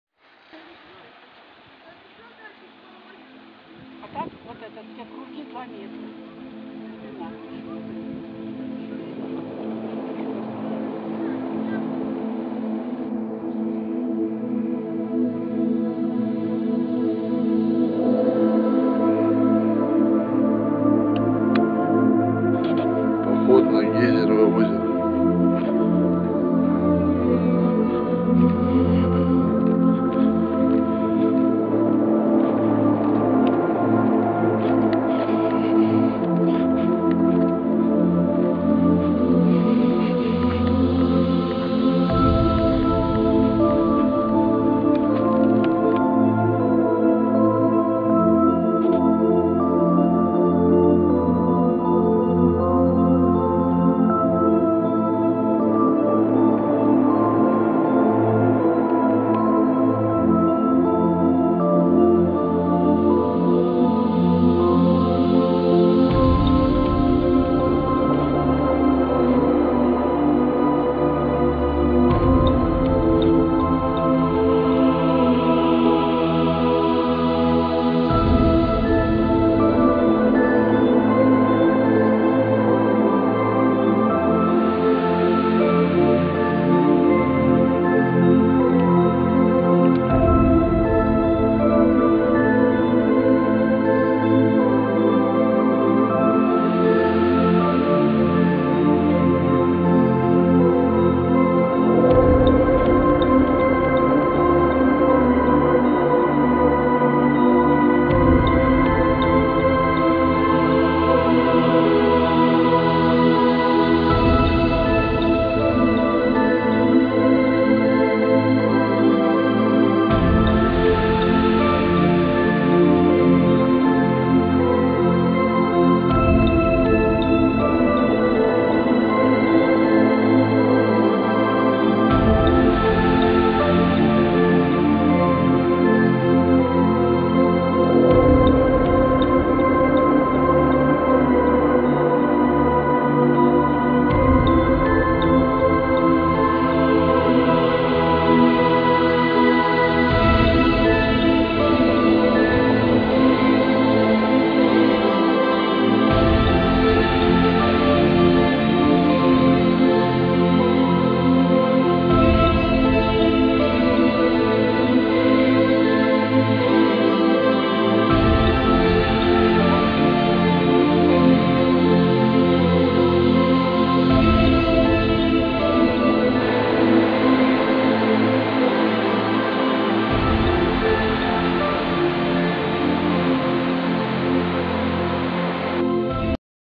Гейзерное озеро на Алтае
Алтай - Горный Алтай - 2016 .
Гейзерное  (Голубое) озеро Улаганский район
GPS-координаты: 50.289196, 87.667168
Природа Алтая. Путешествия, поездки на автомобиле по красотам Горного Алтая.
The Nature Of Altai. Travel, car trips on the beauty of the Mountain Altai.